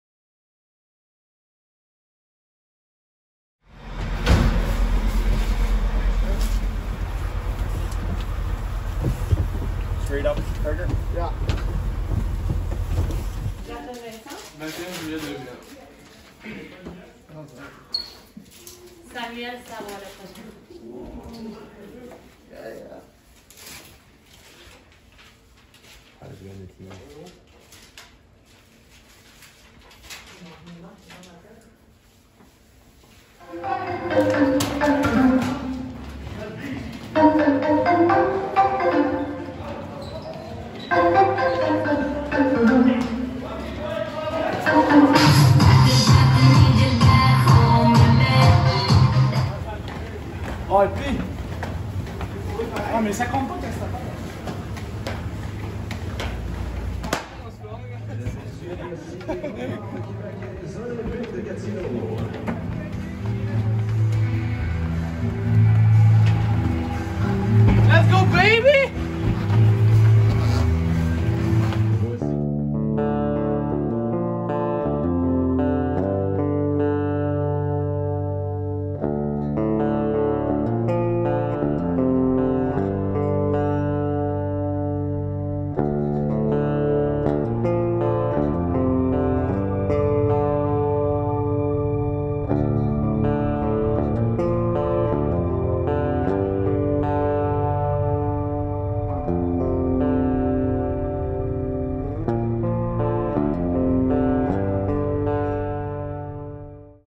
Jour 10
Avant dernière journée dans l'environnement protégé.

Défaite de 2-1 contre les Remparts. Voici un petit récapitulatif de notre journée.